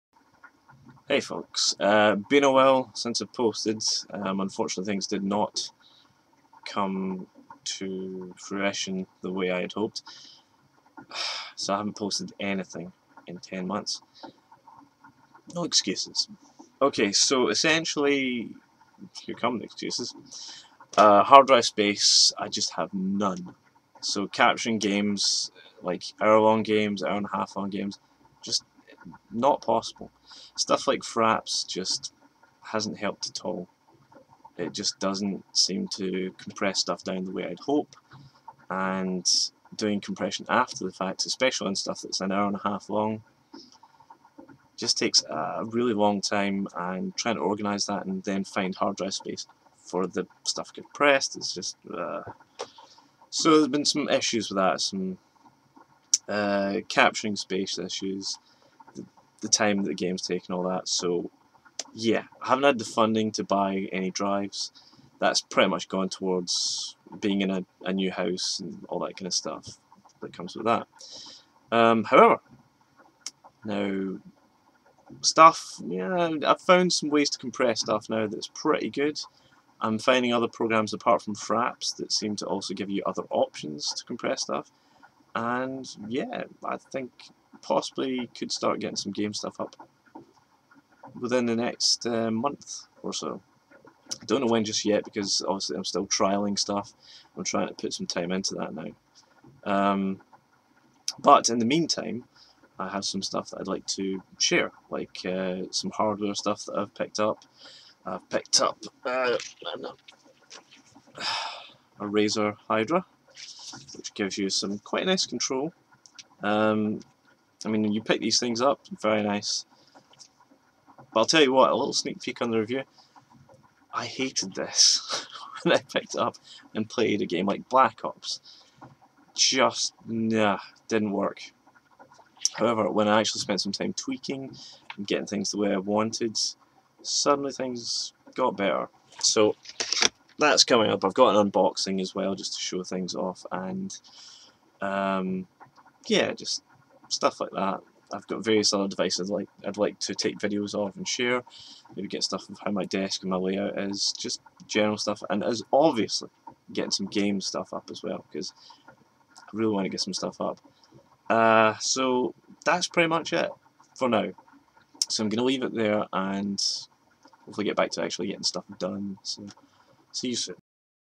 Gaming channel coming back!
A video explaining about the channel and some woes, etc.

Oh dear, oh dear! Yes, I didn't follow through on posting some gaming stuff. Mainly down to hard drive space, video capturing and time. My apologies. However, I hope to try and put some stuff up soon. I've got some hardware I'd like to share. Some with unboxing and others being long time companions! I'll try and post my latest acquisition this coming week.